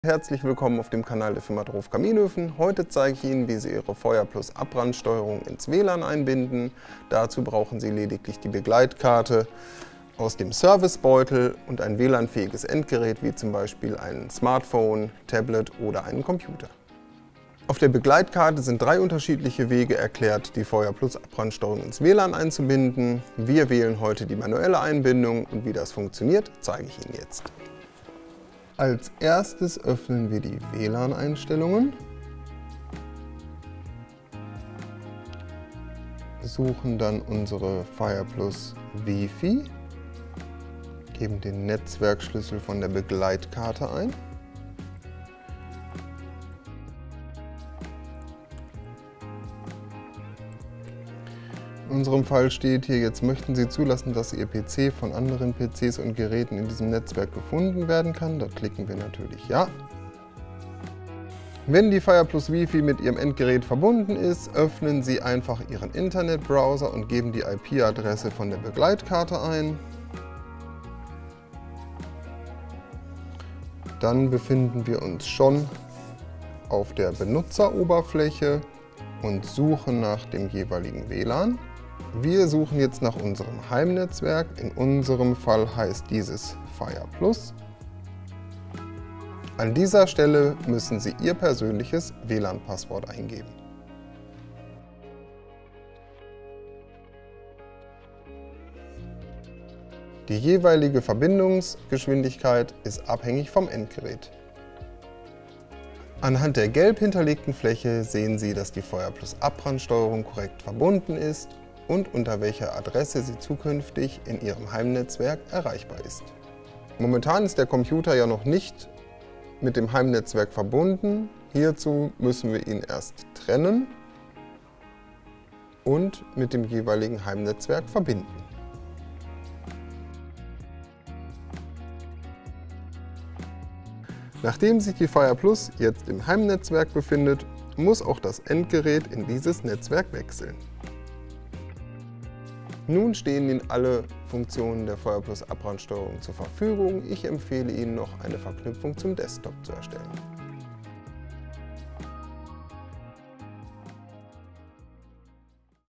Schritt-für-Schritt Anleitung - Einrichtung Abbrandsteuerung fire + im WLAN
Die elektronische Abbrandsteuerung fire+ von DROOFF - wir zeigen Ihnen Schritt für Schritt, wie Sie diese ganz einfach im Heimnetzwerk einbinden!

Sie haben sich für einen DROOFF Kaminofen mit fire+ Abbrandsteuerung entschieden? Damit Sie ganz schnell die zahlreichen Vorzüge auf Knopfdruck genießen können, zeigt Ihnen unser Fachmann Jens, wie Sie mithilfe der beiliegenden WLAN-Begleitkarte Ihre fire+ in nur wenigen Minuten im WLAN einrichten können. Leicht verständlich führt Sie Jens dazu durch den Installationsprozess.

Nach erfolgreicher Einrichtung heißt es Anfeuern, Einkuscheln und der automatischen Abbrandsteuerung fire+ bei der Arbeit zusehen. Der Luftschieber bewegt sich wie von Zauberhand in die optimale Position. Zeit zum Nachlegen? Kein Problem, Sie werden automatisch zum richtigen Zeitpunkt erinnert.

Freuen Sie sich auf kuschelige Stunden vor dem Kaminofen mit Emissionswerten und Wirkungsgrad auf Spitzenniveau und senken Sie gleichzeitig Ihren Holzverbrauch!

Weitere Infos zur Funktionsweise unserer fire+ Abbrandsteuerung finden sie auf unserer offiziellen Hersteller oder in unserem Herzglut-Magazin